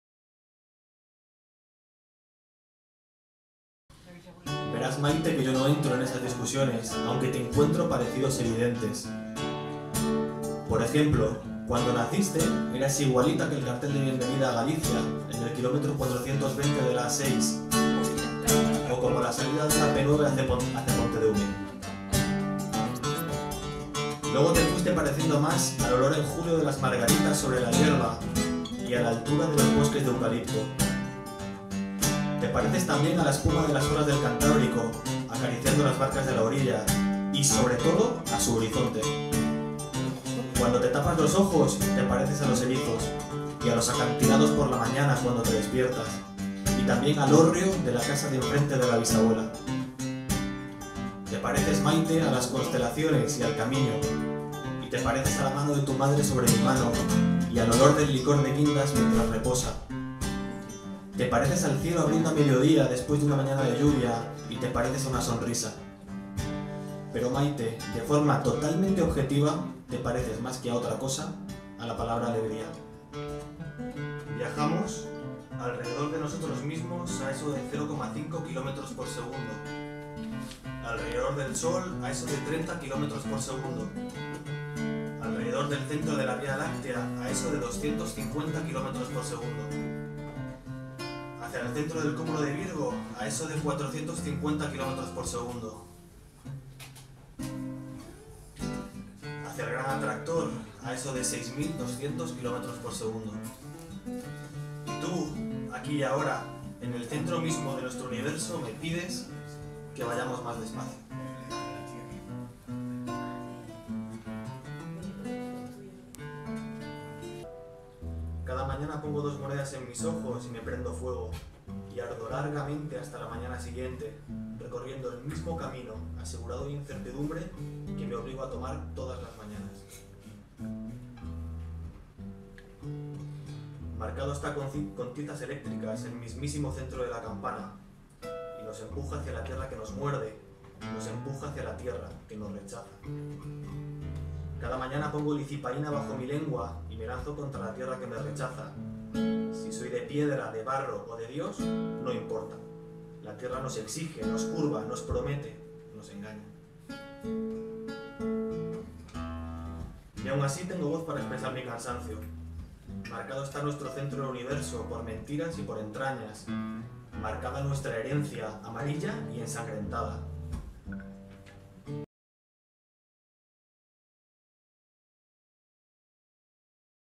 Tomás G. Purriños y Álvaro Miranda en Mursiya Poética
Resumen del recital que el poeta Tomás García Purriños y el músico Álvaro Miranda dieron el Viernes 9 de mayo de 2014 dentro del ciclo Mursiya Poética en La Azotea (Murcia).